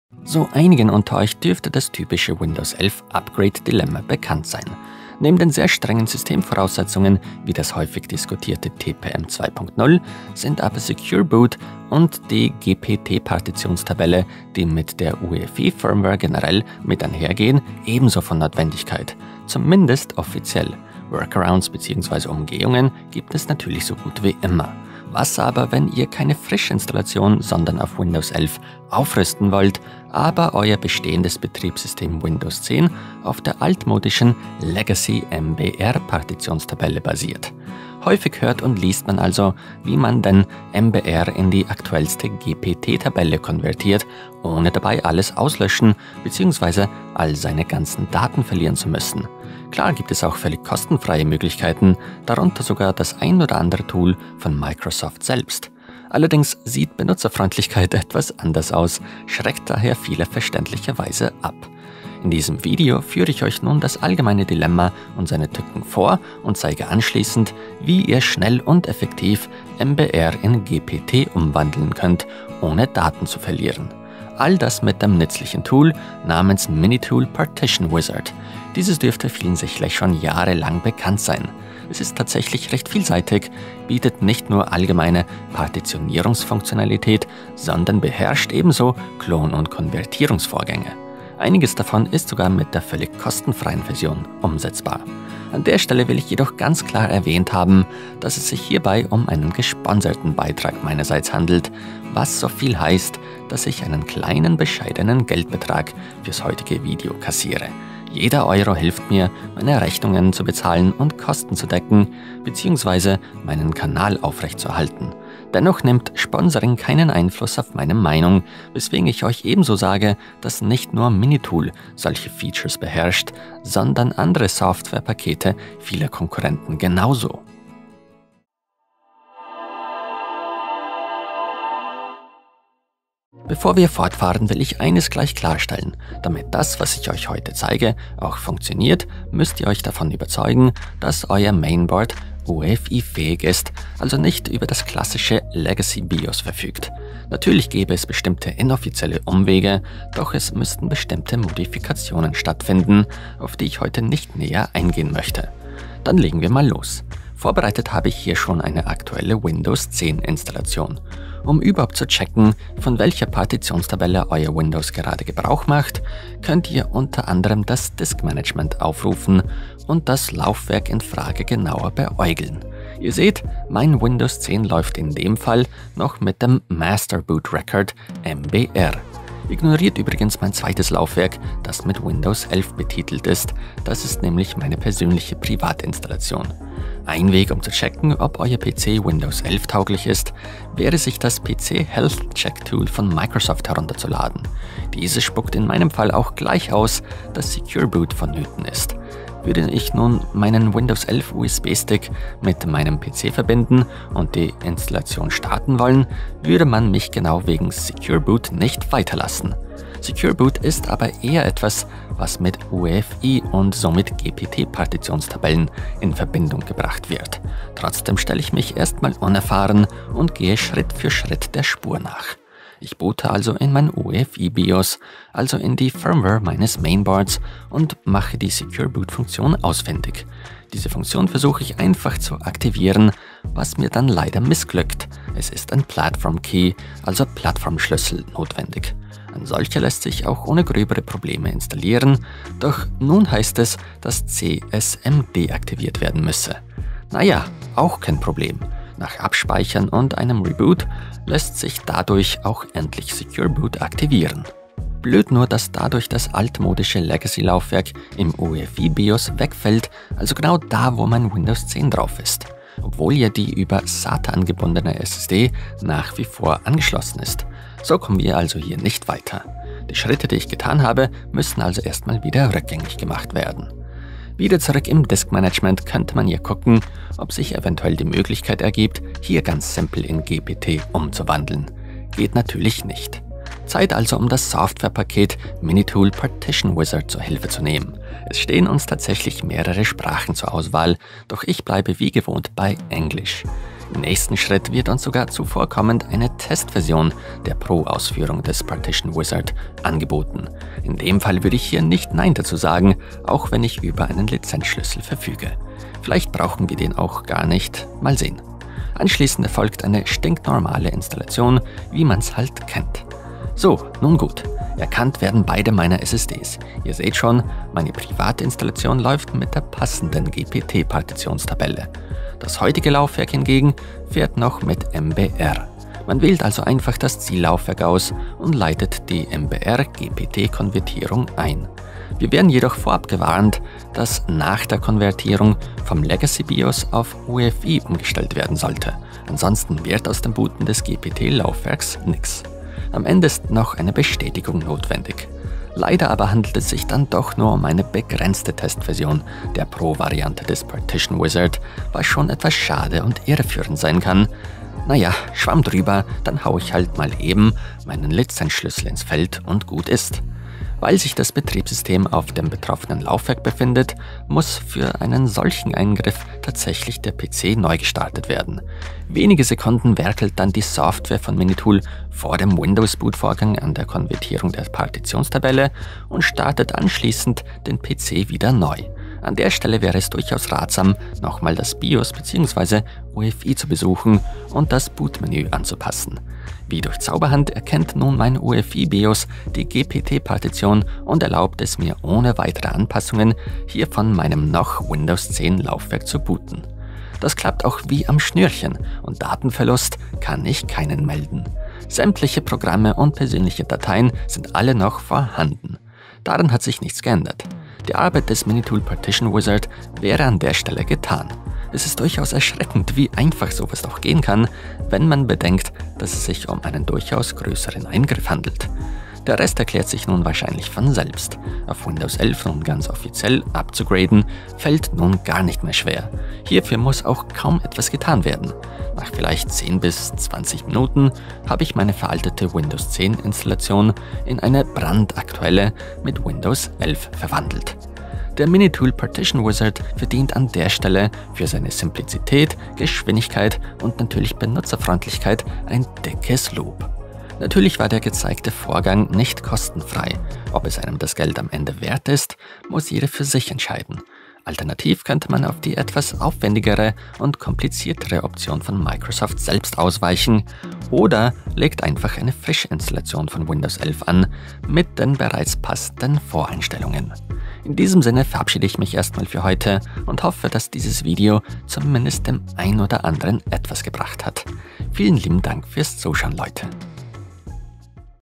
MBR in GPT konvertieren OHNE Datenverlust -- MiniTool Partition Wizard
⚠✅❗WERBUNG❗✅⚠
In diesem Video zeige ich euch, wie ihr MBR in GPT konvertieren könnt, sodass ihr erfolgreich auf Windows 11 aufrüsten könnt ohne dabei eure Daten zu verlieren.